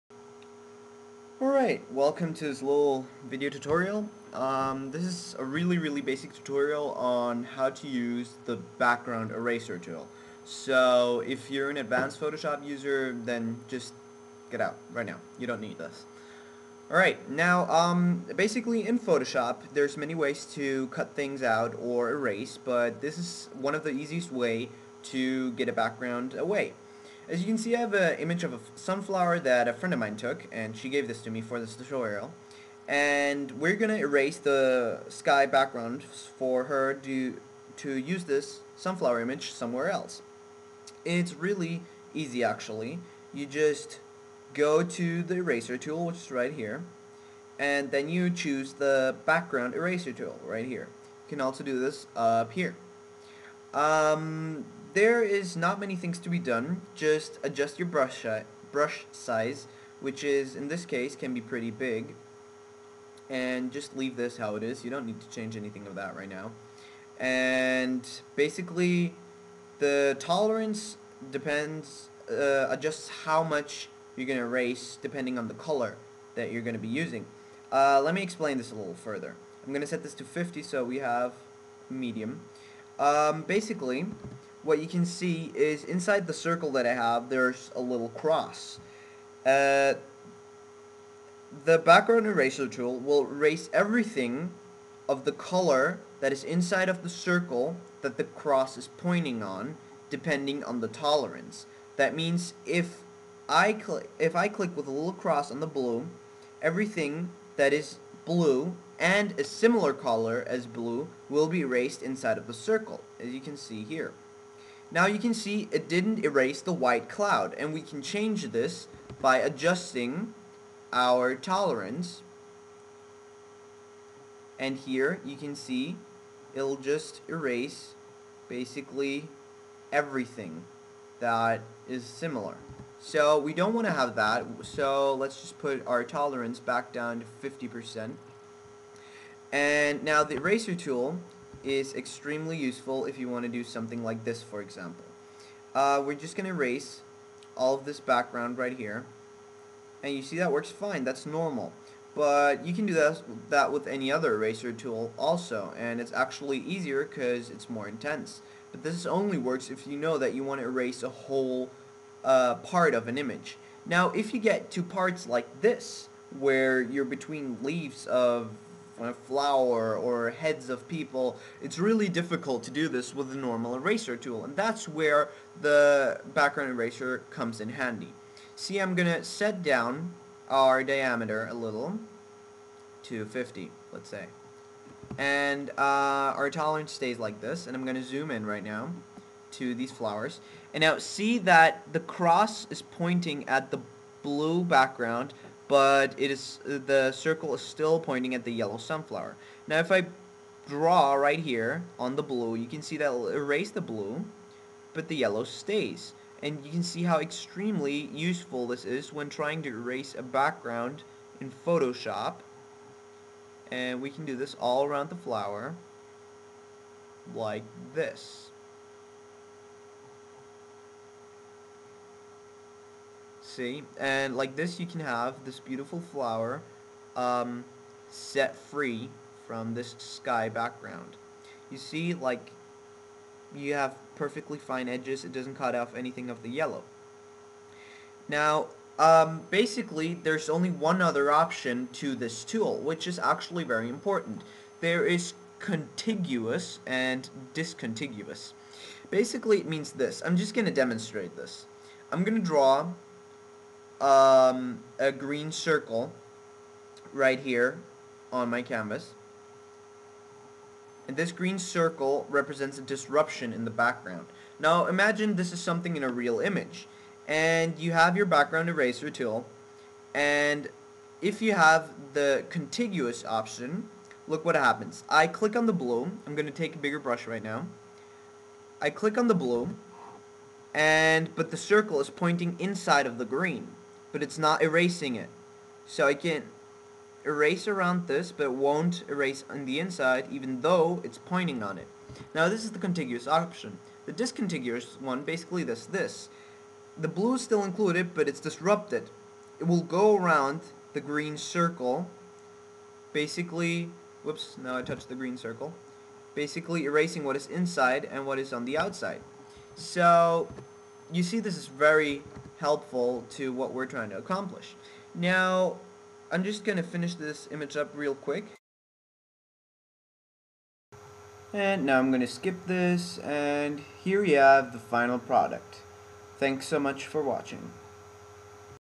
Basic Photoshop Tutorial: Background Eraser
A very Basic Tutorial, showing how to use the background eraser tool to get rid of the backgroud. Done with Photoshop Elements.